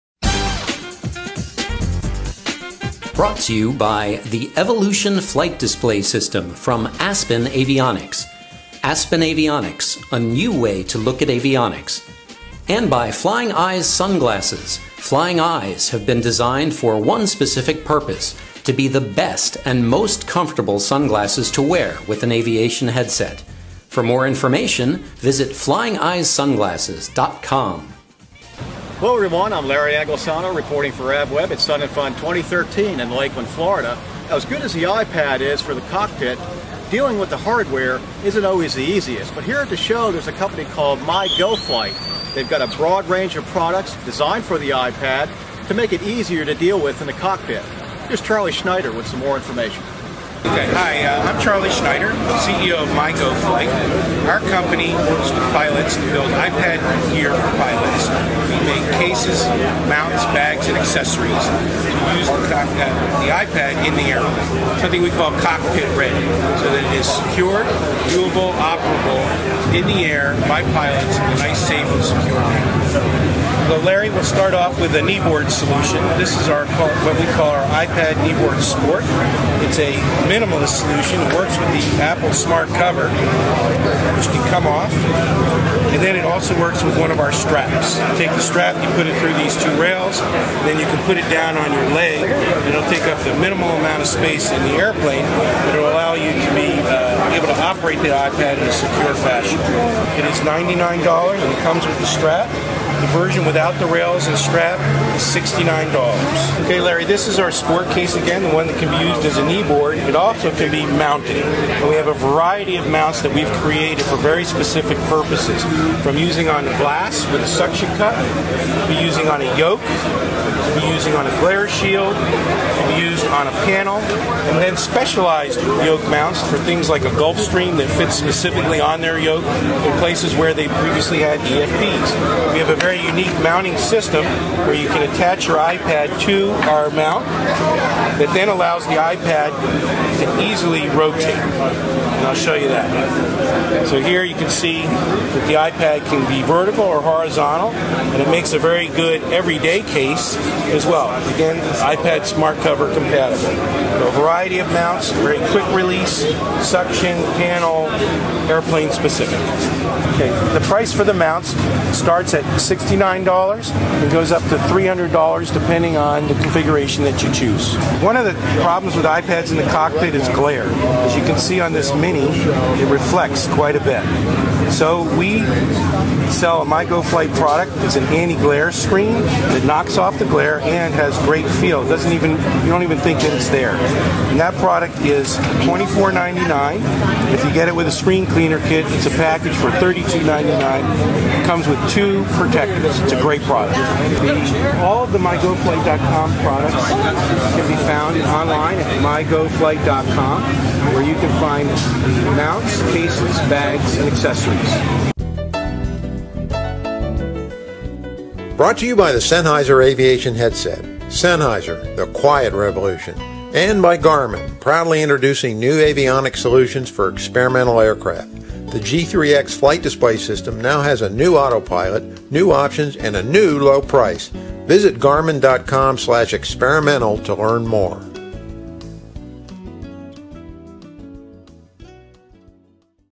MyGoFlight iPad Cockpit Accessories
Without the proper hardware, managing an iPad in the cockpit can be a challenge. MyGoFlight has a line of high-quality iPad mounts, knee boards, cases and other accessories designed for the task. AVweb's Larry Anglisano had a look at the products at Sun 'n Fun 2013 in Lakeland, Florida.